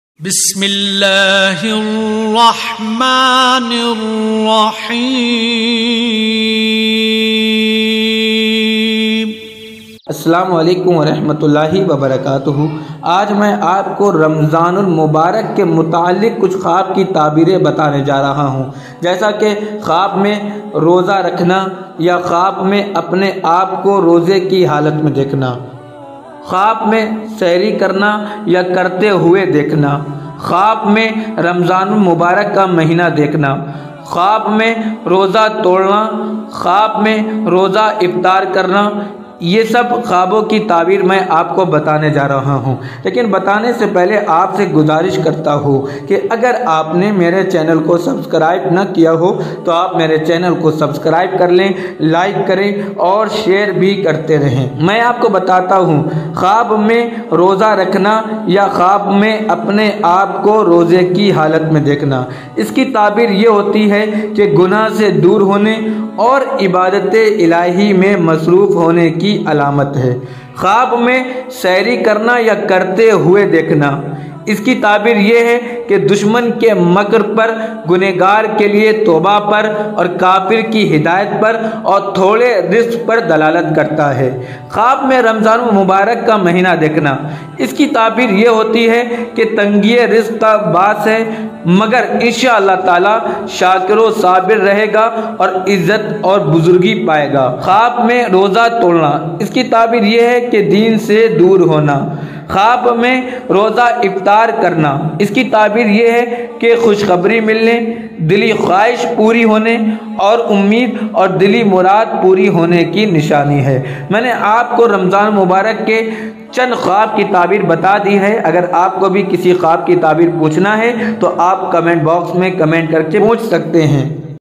Khawab me Roza Rakhne ki Tabeer Khawb me saheri ya iftaar karna ख्वाब में रोजा इफ्तार रमज़ान देखना
CHANNEL NAAM

TARIQI ISLAMICE KNOWLEDGE